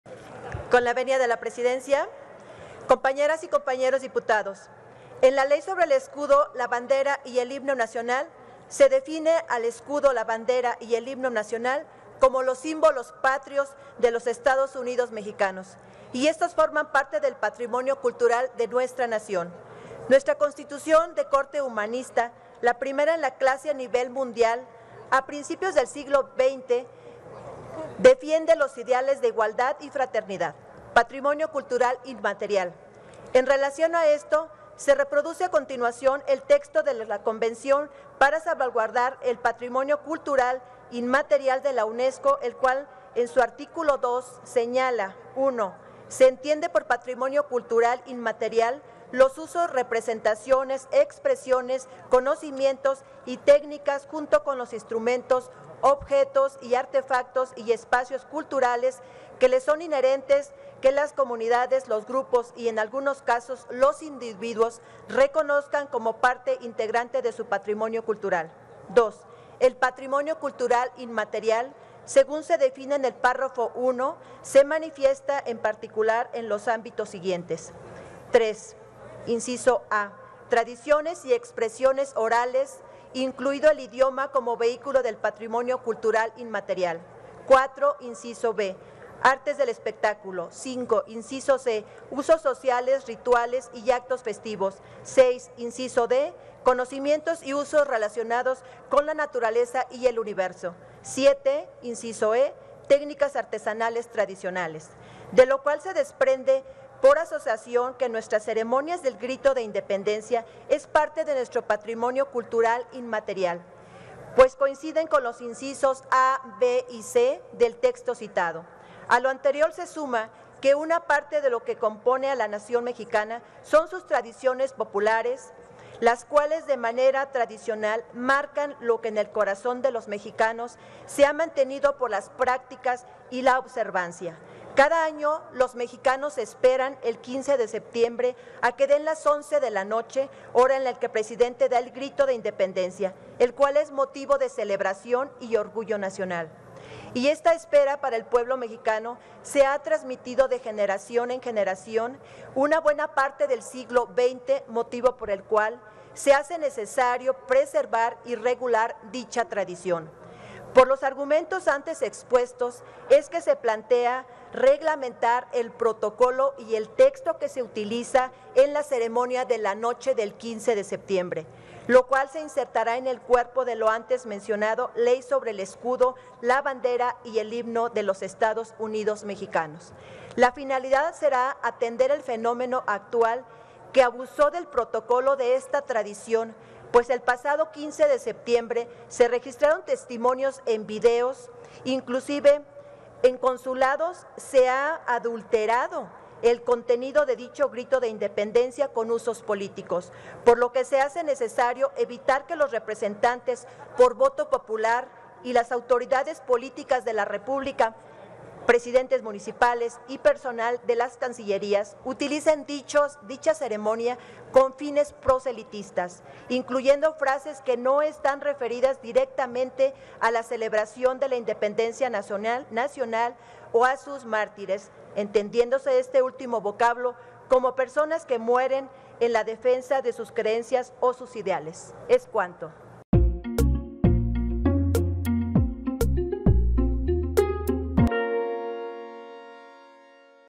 Dip. Macarena Chávez (PRD) - Iniciativa reglamentar protocolo en ceremonia del 15 de septiembre
La diputada María Macarena Chávez Flores (PRD) presenta una iniciativa que reforma la Ley sobre el Escudo, la Bandera y el Himno Nacionales, a fin de reglamentar el protocolo y el texto que se utilizará en la ceremonia de la noche del 15 de septiembre.